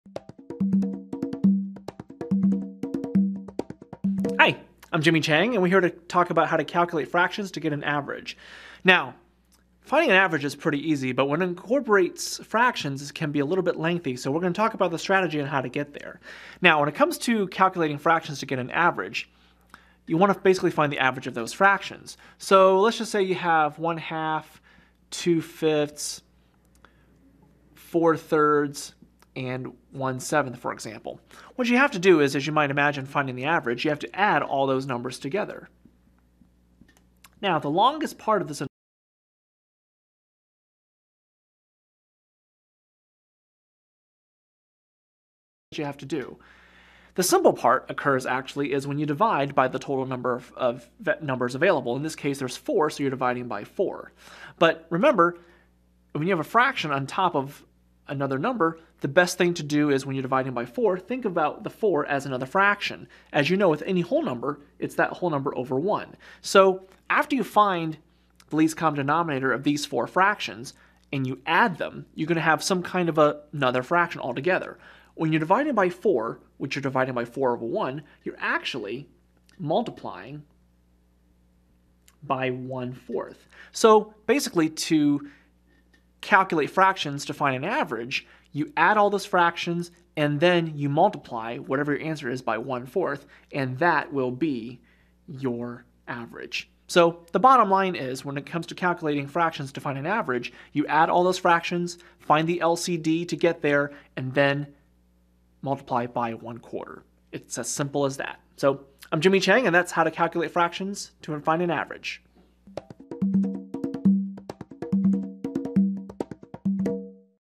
How to Calculate Fractions to Get an Average
How to Calculate Fractions to Get an Average. Part of the series: Math Problems. Finding the average of fractions is a pretty easy, albeit lengthy, process. Calculate fractions to get an average with help from a mathematics educator in this free video clip.